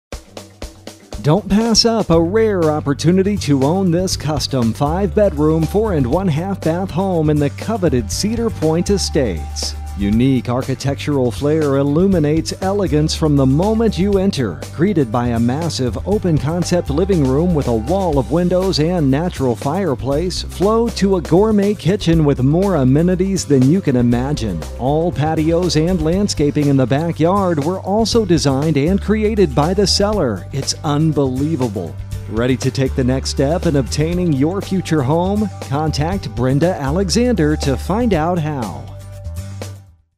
5249 Red Cedar Court West Bend, WI | ColdwellBankerHomes.com
5249 Red Cedar Court, West Bend, WI

Rare opportunity to offer a unique custom original in the coveted Cedar Point Estates! This one SCREAMS entertaining! Unique architectural flair illuminates elegance from the moment you enter. Greeted by a massive, open concept living room with wall of windows, natural fireplace, flows to a gourmet